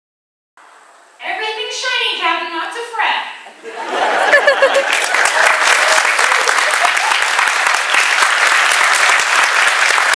Jewel Staite " Shiny"
Space City Con 2013  Jewel Staite was ask to say a line as Kaylee , I got her saying it but sadly the video didn't auto focus enough for you see it well.